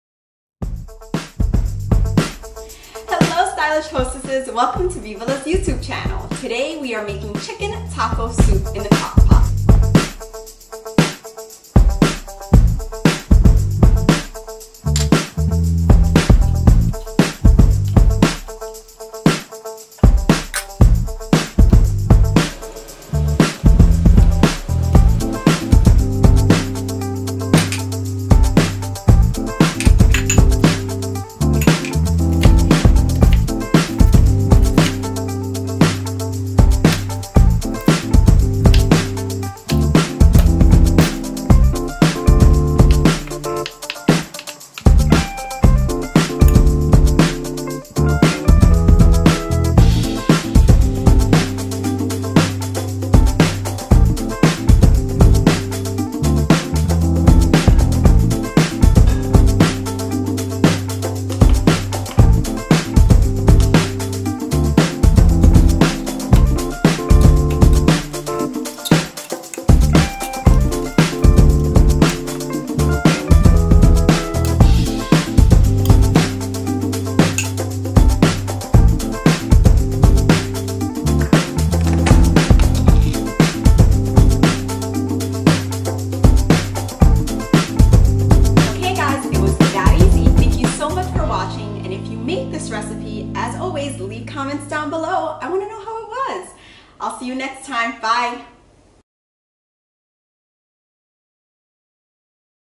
Master the Crockpot With a Delicious Chicken Taco Soup
Our stylish hostess Kathleen Pagan makes a delicious chicken taco soup using a crockpot. Visit Vivala.com for the recipe and five tips for mastering the perfect kitchen tool for the woman on the go